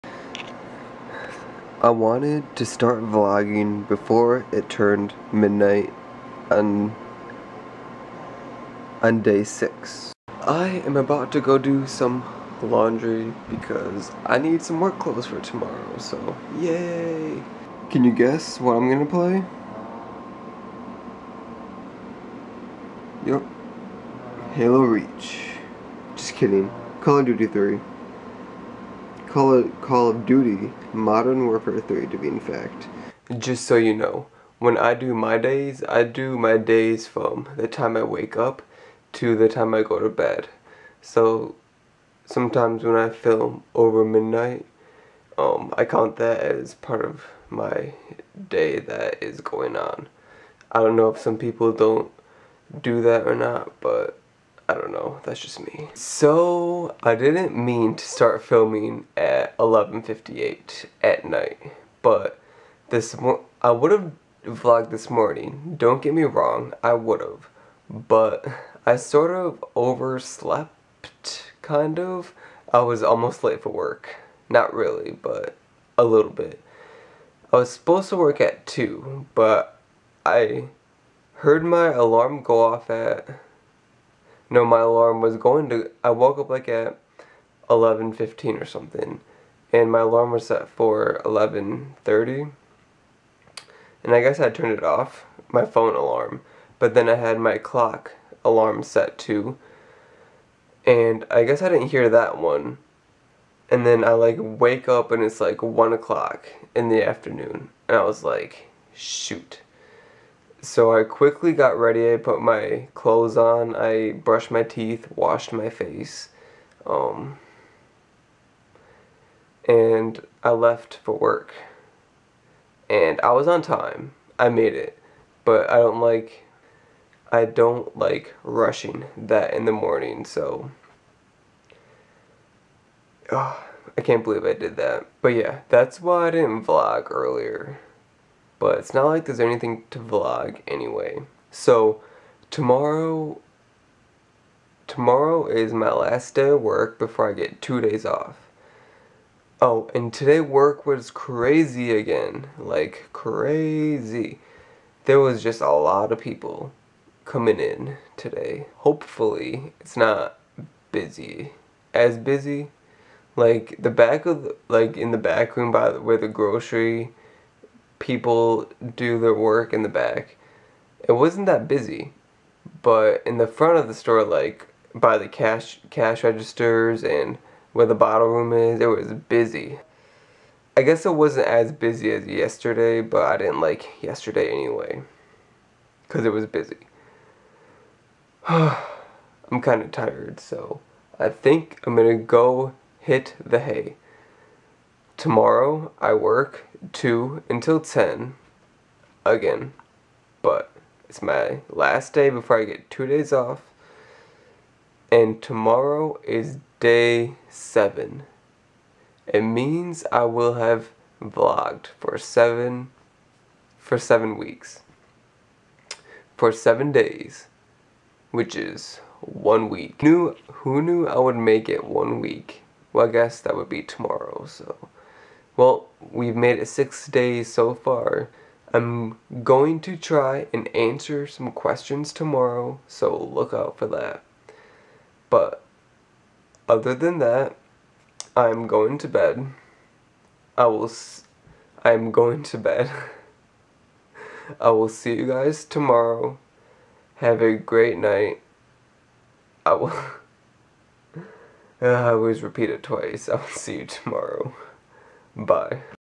Almost Late for Work (08/06/12) Day #6
I sort of sleep in.